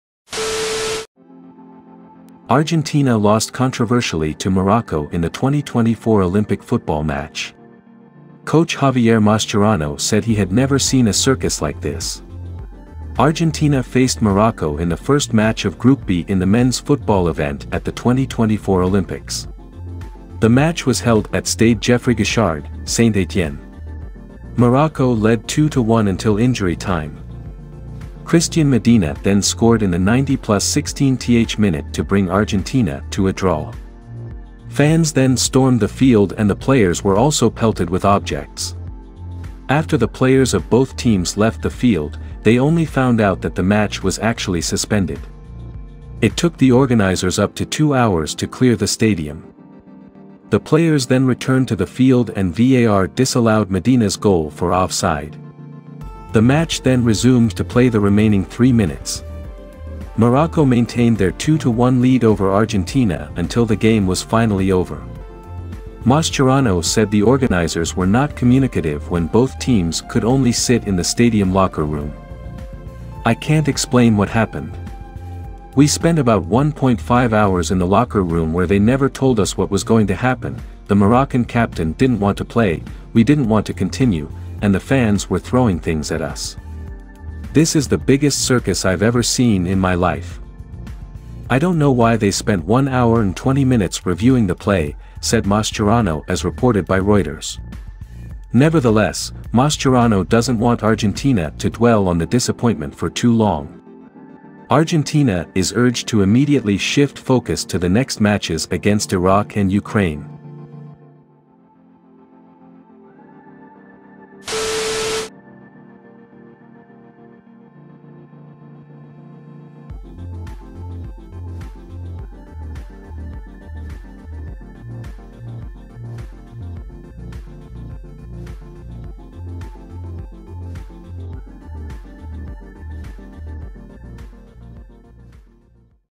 ARGENTINA LOSES WITH CONTROVERSY MASCHERANO CALLS IT A CIRCUS!!!
Argentina lost controversially to Morocco in the 2024 Olympic football match. Coach Javier Mascherano said he had never seen a circus like this. Argentina faced Morocco in the first match of Group B in the men's football event at the 2024 Olympics. The match was held at Stade Geoffroy-Guichard, Saint-Etienne. Morocco led 2-1 until injury time. Cristian Medina then scored in the 90+16th minute to bring Argentina to a draw. Fans then stormed the field and the players were also pelted with objects. After the players of both teams left the field, they only found out that the match was actually suspended. It took the organizers up to two hours to clear the stadium. The players then returned to the field and VAR disallowed Medina's goal for offside. The match then resumed to play the remaining three minutes. Morocco maintained their 2-1 lead over Argentina until the game was finally over. Mascherano said the organizers were not communicative when both teams could only sit in the stadium locker room. "I can't explain what happened. We spent about 1.5 hours in the locker room where they never told us what was going to happen, the Moroccan captain didn't want to play, we didn't want to continue, and the fans were throwing things at us. This is the biggest circus I've ever seen in my life. I don't know why they spent 1 hour and 20 minutes reviewing the play," said Mascherano as reported by Reuters. Nevertheless, Mascherano doesn't want Argentina to dwell on the disappointment for too long. Argentina is urged to immediately shift focus to the next matches against Iraq and Ukraine.